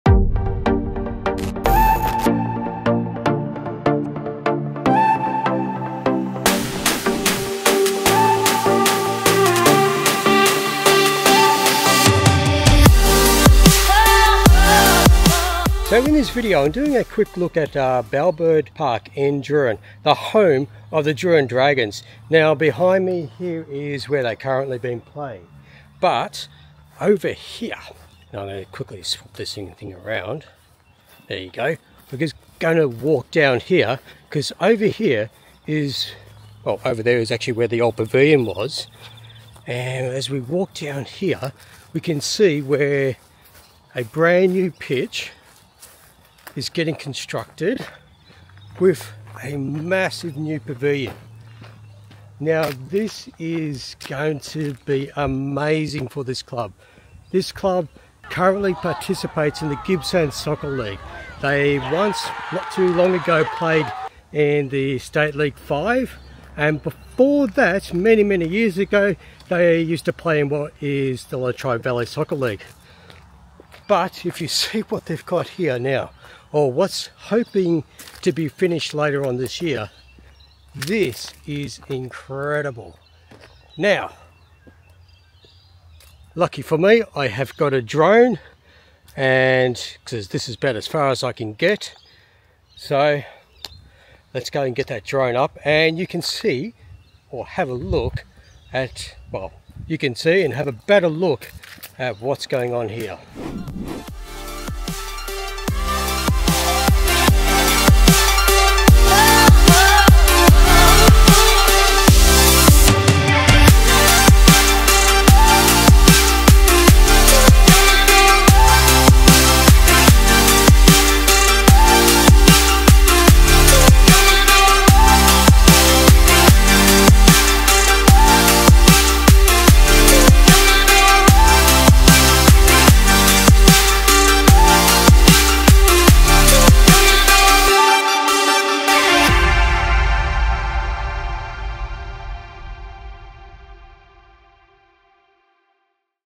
INCREDIBLE! : Bellbird Park, home of the Drouin Dragons...
Drouin Dragons are a club that fields teams in the Gippsland Soccer League and very soon they will be the envy of many clubs that even particpate in the State Leagues!